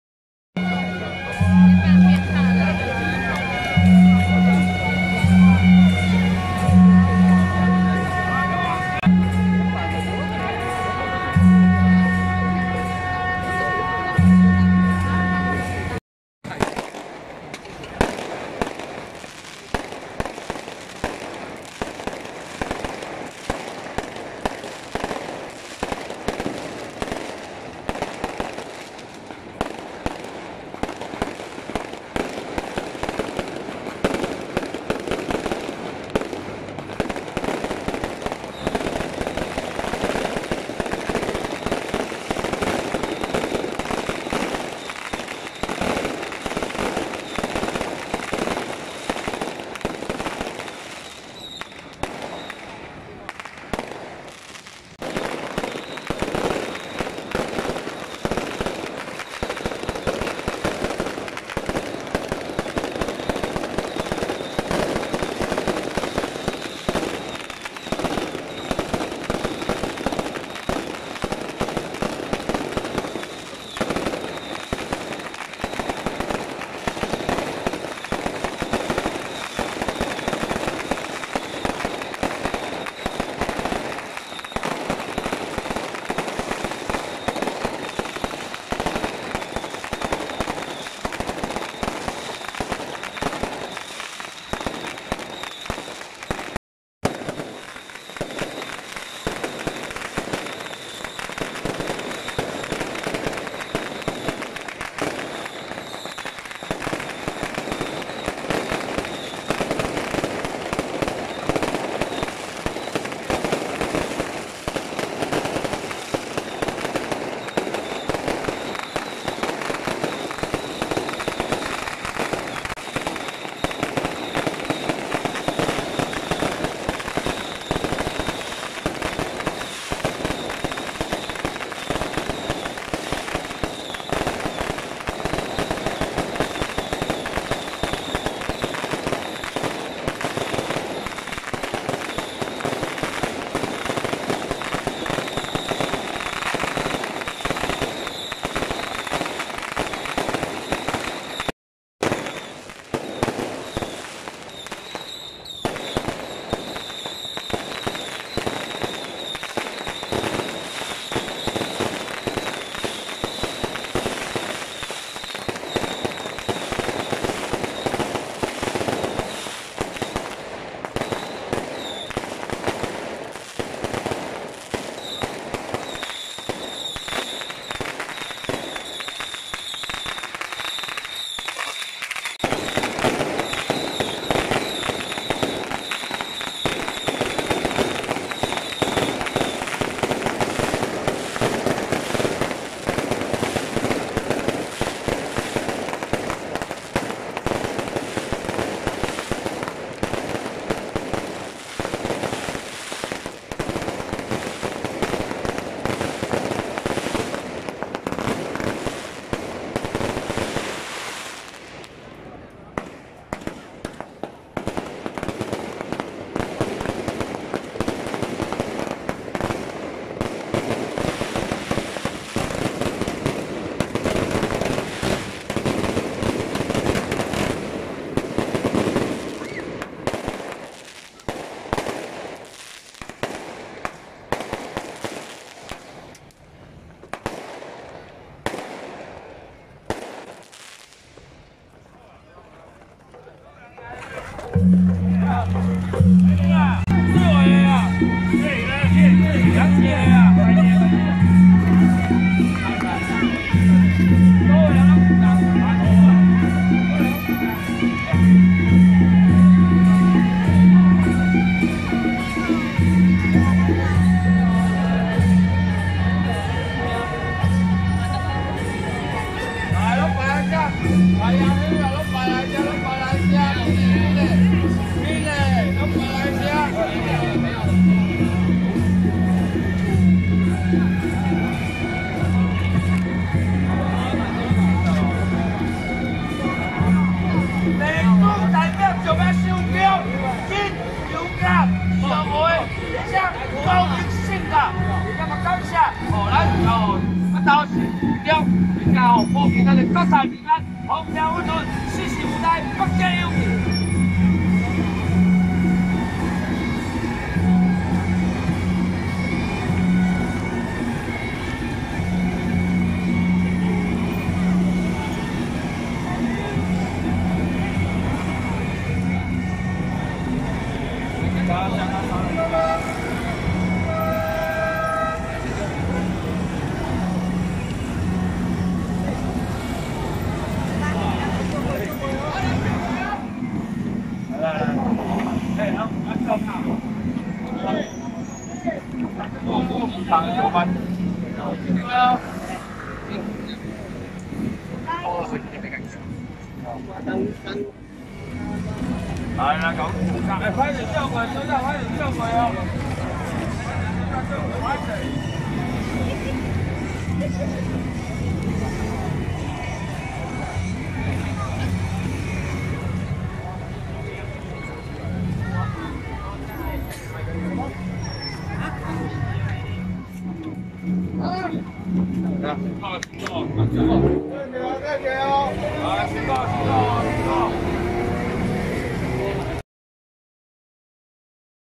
【石牌聖雲宮 炮贊 北投新武宮新武獅】~109北投新武宮新武獅白鶴先師南巡謁祖會香回駕遶境
石牌聖雲宮 炮贊 北投新武宮新武獅